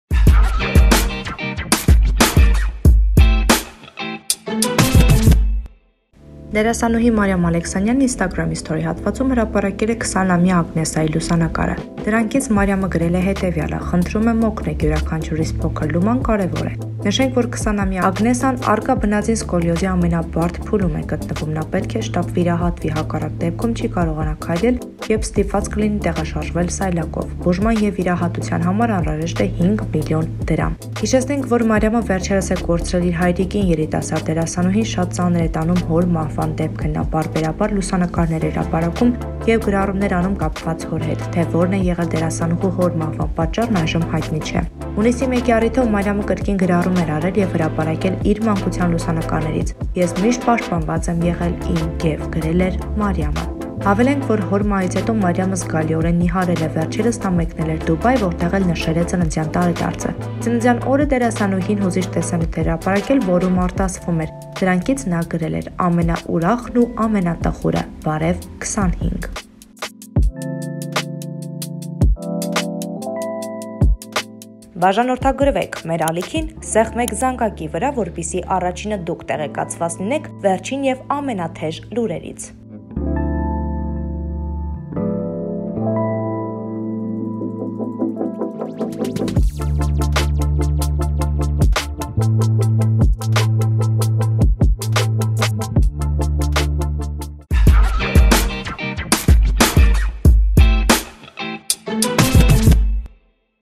Մարիամ Ալեքսանյանն օգնության կարիք ունի
Մարիամը բավական փակ է պահում իր անձնական կյանքը: Մինչ այդ նա նշանադրված էր դերասան Գոռ Համբարձումյանի հետ: Նրանք միանում և բաժանվում էին, սակայն ի վերջո նշանադրությունը չեղարկելու որոշում կայացրին: Տեւական ժամանակ Մարիամը խուսափում էր որեւէ մեկնաբանություն տալ, նա դա արել էր ինստագրամյան էջում անցկացրած հարց ու պատասխանի ժամանակ, երբ պատասխանել էր հետեւորդներից մեկի հարցին՝ արդյո՞ք նշանված է:

Մանրամասն՝ տեսանյութում